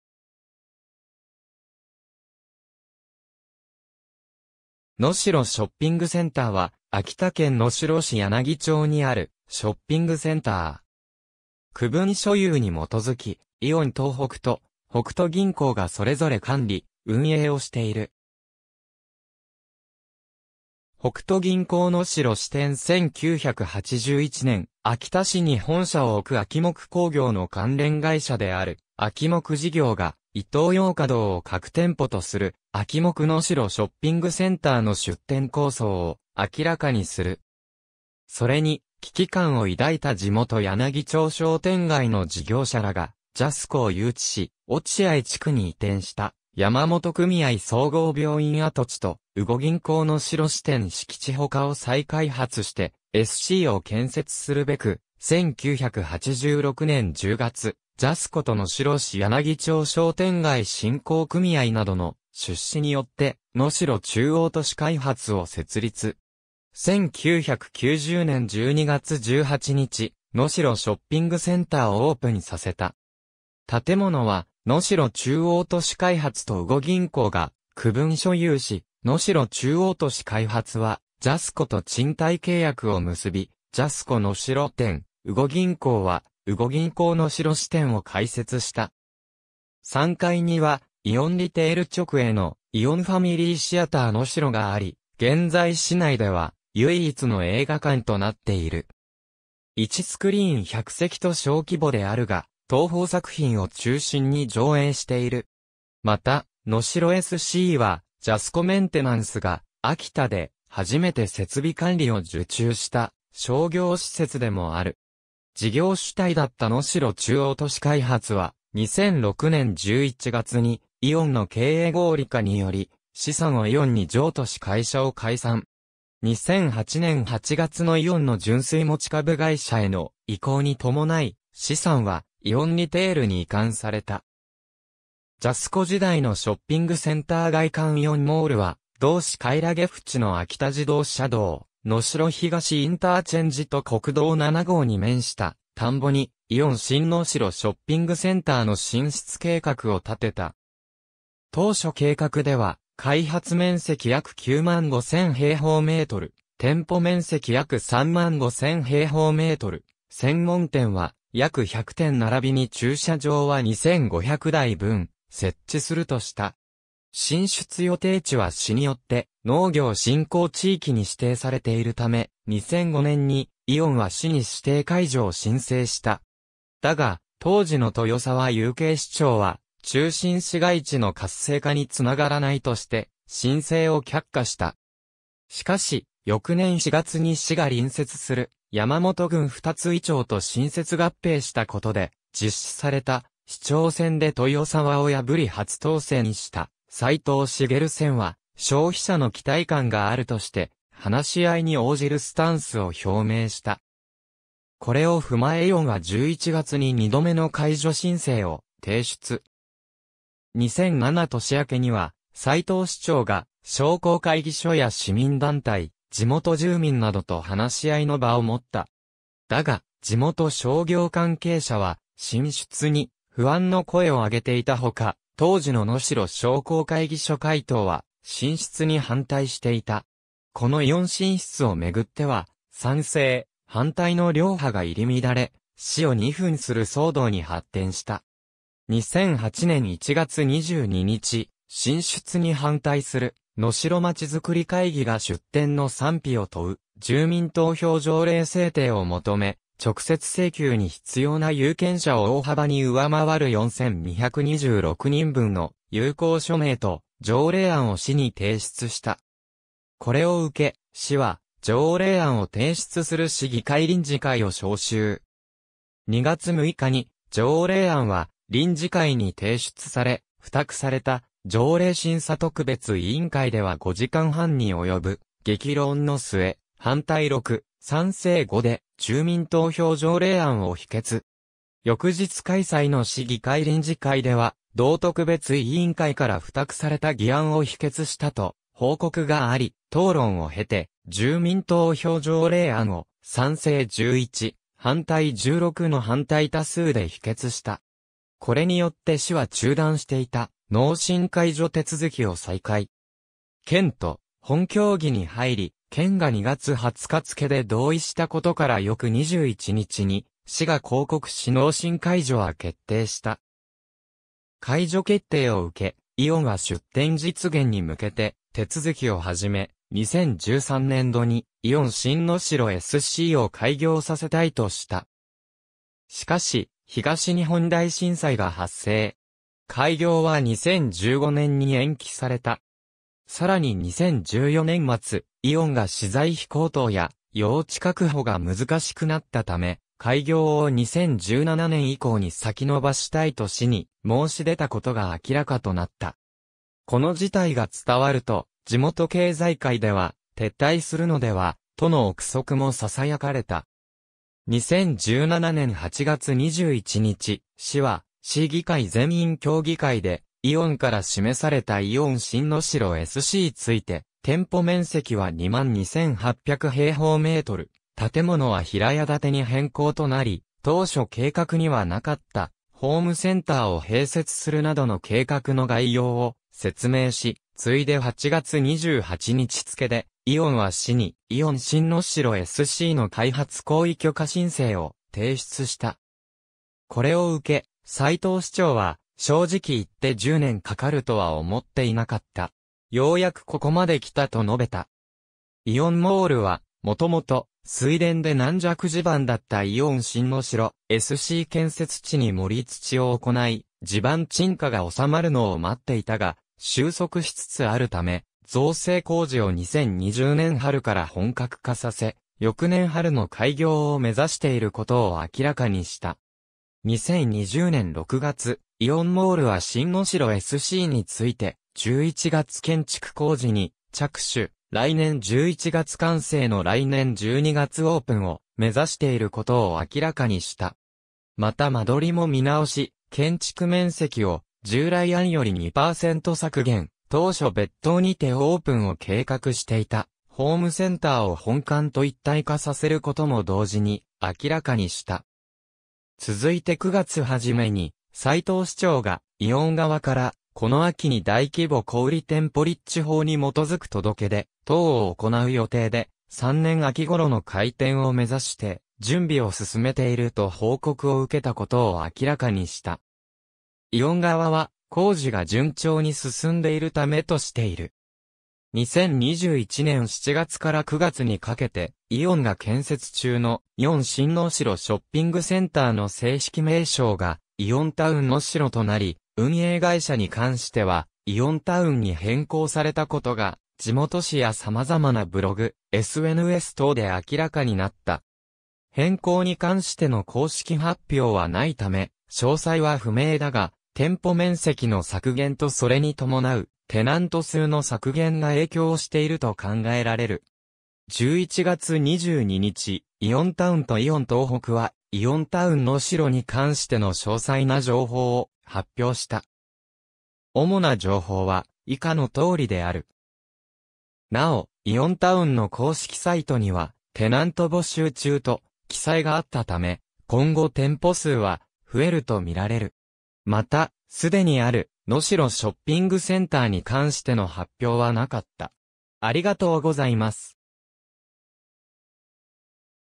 能代ショッピングセンター
能代ショッピングセンター（のしろショッピングセンター）は、秋田県能代市柳町にあるショッピングセンター。
区分所有に基づきイオン東北と北都銀行がそれぞれ管理・運営をしている。
北都銀行能代支店 1981年（昭和56年）、秋田市に本社を置く秋木工業の関連会社である秋木事業がイトーヨーカドーを核店舗とする「秋木能代ショッピングセンター」の出店構想を明らかにする（しかし、秋木工業は1982年11月に倒産。SCも結局開業に至らず）。
それに危機感を抱いた地元柳町商店街の事業者らが、ジャスコ（現：イオン）を誘致し、落合地区に移転した山本組合総合病院跡地と「羽後銀行能代支店」敷地ほかを再開発してSCを建設するべく、1986年（昭和61年）10月、ジャスコと能代市柳町商店街振興組合などの出資によって能代中央都市開発を設立。
1990年（平成2年）12月18日、「能代ショッピングセンター」をオープンさせた。
建物は能代中央都市開発と羽後銀行が区分所有し、能代中央都市開発はジャスコと賃貸契約を結び「ジャスコ能代店」、羽後銀行は「羽後銀行能代支店」を開設した。
3階にはイオンリテール直営の「イオンファミリーシアター能代」（旧：ジャスコファミリーシアター能代）があり、現在市内では唯一の映画館となっている。
1スクリーン100席と小規模であるが、東宝作品を中心に上映している。
また、能代SCはジャスコメンテナンス（現在のイオンディライト）が秋田で初めて設備管理を受注した商業施設でもある。
事業主体だった能代中央都市開発は、2006年（平成18年）11月にイオンの経営合理化により資産をイオンに譲渡し会社を解散。
2008年（平成20年）8月のイオンの純粋持株会社への移行に伴い、資産はイオンリテールに移管された。
ジャスコ時代のショッピングセンター外観 イオンモールは同市鰄渕の秋田自動車道能代東ICと国道7号に面した田んぼに「イオン新能代ショッピングセンター（仮称）」の進出計画を立てた。
当初計画では、開発面積約95,000m2、店舗面積約35,000m2、専門店は約100店ならびに駐車場は2500台分設置するとした。
進出予定地は市によって農業振興地域に指定されているため、2005年（平成17年）にイオンは市に指定解除を申請した。
だが当時の豊澤有兄市長は「中心市街地の活性化につながらない」として申請を却下した。
しかし、翌年4月に市が隣接する山本郡二ツ井町と新設合併したことで実施された市長選で豊澤を破り初当選した斉藤滋宣は、「消費者の期待感」があるとして、話し合いに応じるスタンスを表明した。
これを踏まえイオンは11月に2度目の解除申請を提出。
2007年（平成19年）明けには、斉藤市長が商工会議所や市民団体、地元住民などと話し合いの場を持った。
だが、地元商業関係者は進出に不安の声を上げていたほか、当時の能代商工会議所会頭は進出に反対していた。
このイオン進出を巡っては、賛成、反対の両派が入り乱れ、市を二分する騒動に発展した。
2008年（平成20年）1月22日、進出に反対する「能代まちづくり会議」が出店の賛否を問う住民投票条例制定を求め、直接請求に必要な有権者を大幅に上回る4226人分の有効署名と条例案を市に提出した。
これを受け、市は条例案を提出する市議会臨時会を招集。
2月6日に条例案は臨時会に提出され、負託された条例審査特別委員会では5時間半に及ぶ激論の末、反対6、賛成5で住民投票条例案を否決。
翌日開催の市議会臨時会では同特別委員会から付託された議案を否決したと報告があり、討論を経て、住民投票条例案を、賛成11、反対16の反対多数で否決した。
これによって市は中断していた農振解除手続きを再開。
県と本協議に入り、県が2月20日付で同意したことから翌21日に市が公告し農振解除は決定した。
解除決定を受け、イオンは出店実現に向けて手続きを始め、2013年（平成25年）度にイオン新能代SC（仮称）を開業させたいとした。
しかし、東日本大震災が発生。
開業は2015年（平成27年）に延期された。
さらに2014年（平成26年）末、イオンが資材費高騰や用地確保が難しくなったため、開業を2017年（平成29年）以降に先延ばしたいと市に申し出たことが明らかとなった。
この事態が伝わると地元経済界では「撤退するのでは」との憶測も囁かれた。
2017年8月...